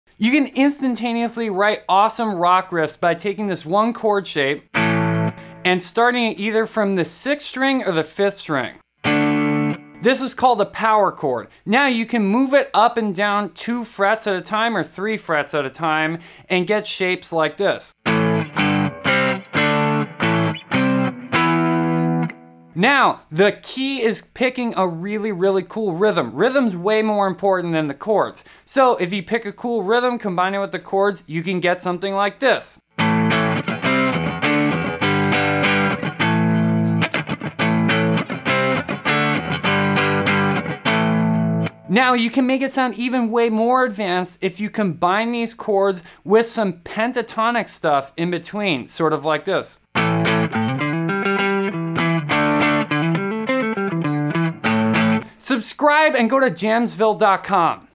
Write EPIC Rock Guitar Riffs In 1 Minute!
Use the power chord shape, and add in a cool rhythm to make awesome rock riffs almost instantly!

MY GEAR:
Vocal Microphone
Guitar Microphone